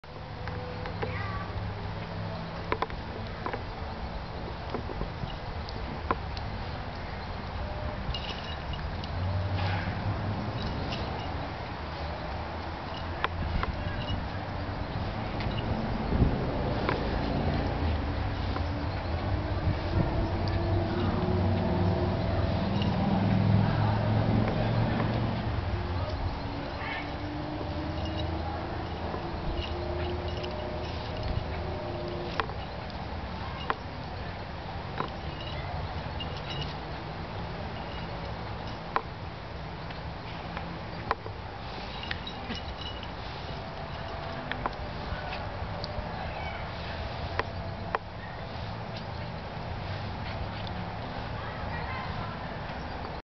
Valentino, Jack russell terrier playing with water 3
Valentino, my jack russell terrier playing with the water in the backyard, jumping and doing semi back flips..part 3rd